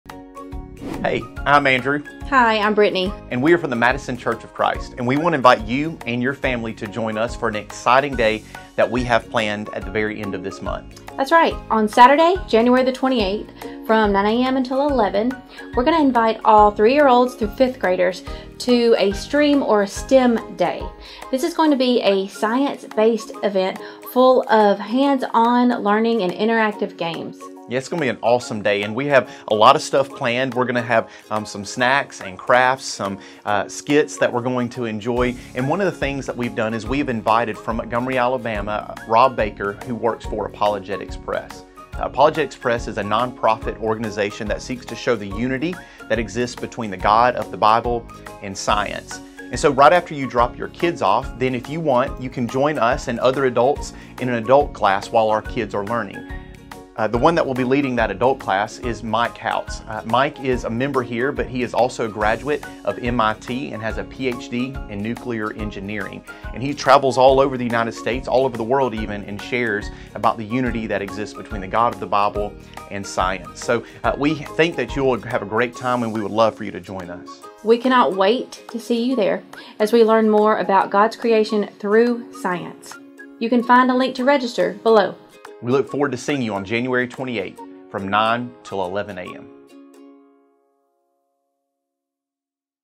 2023 Designed & Created Day
Designed & Planned is a day for families to learn more about the wonder and unity of the Bible and science. Join us on January 28th from 9am until 11am for a day of interactive learning! We will also be providing an adult session taught by NASA Nuclear Engineer, Dr. Mike Houts.